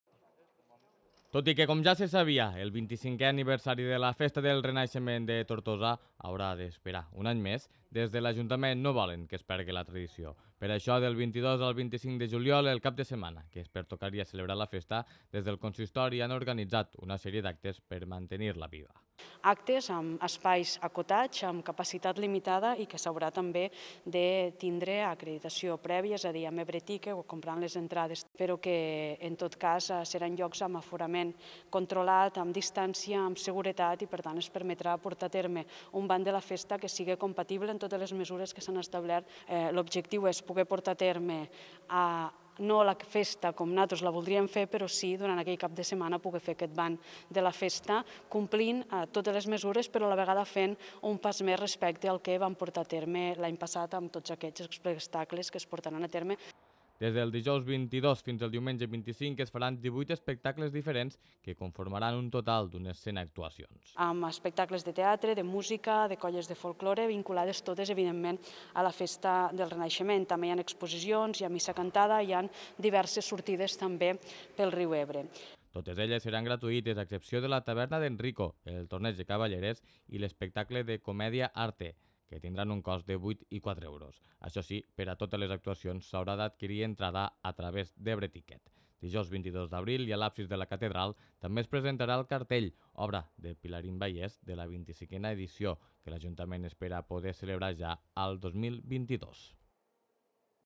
Tortosa organitza un Ban de la Festa del Renaixement amb prop d'un centenar d'actes
Del 22 al 25 de juliol, l'esperit del Renaixement es viurà a carrers i places del nucli antic de Tortosa. No serà, però, en el format habitual de la Festa del Renaixement: les mesures vigents per la situació de pandèmia que encara segueix afectant les activitats que es poden dur a terme fan inviable poder organitzar una edició com les dels anys anteriors